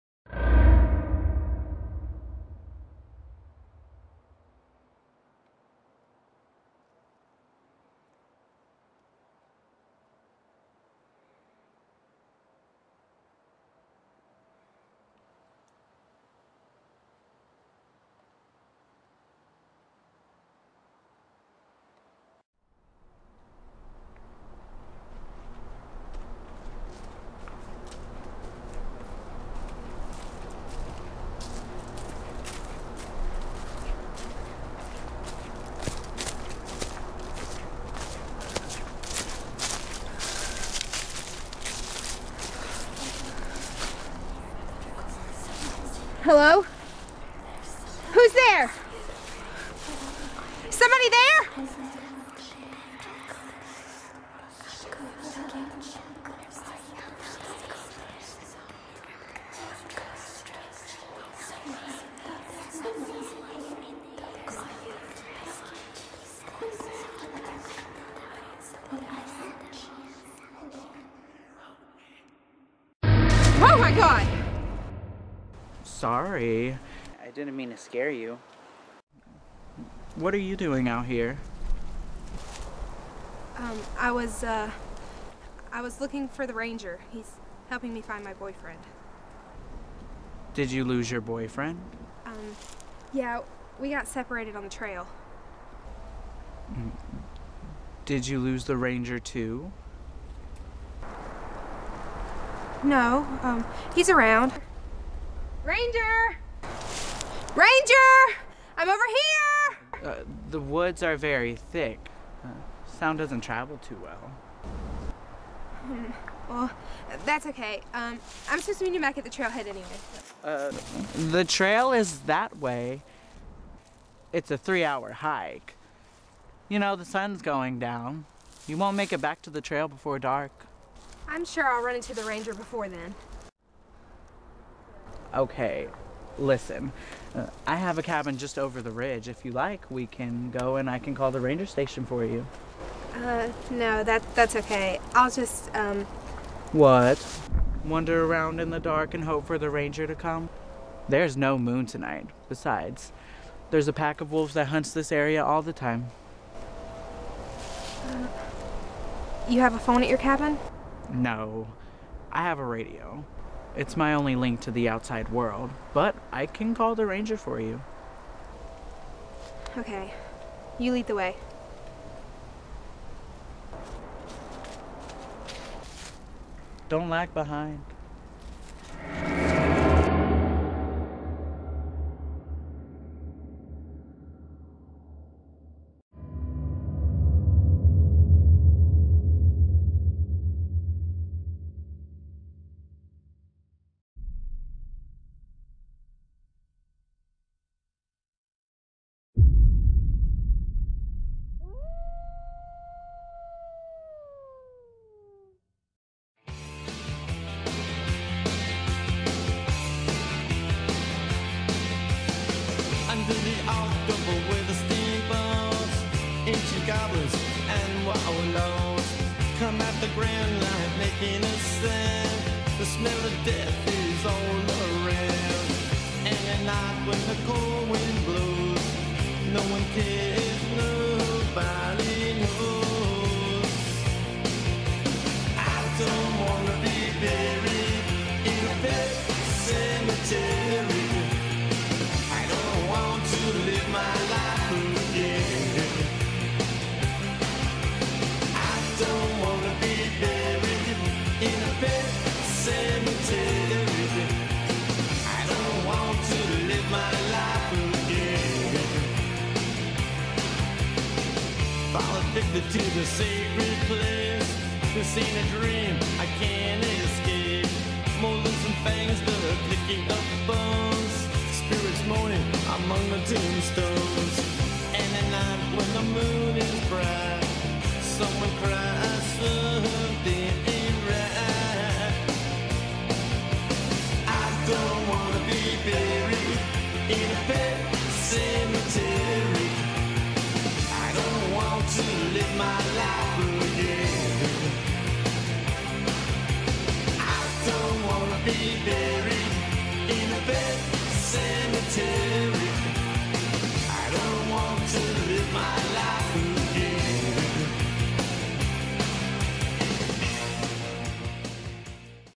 ENCOUNTER IN THE WOODS
A scene produced for Armstrong State University's course Acting for the Camera starring Theatre Performance major Kyndall Hagan and JoJo Ward.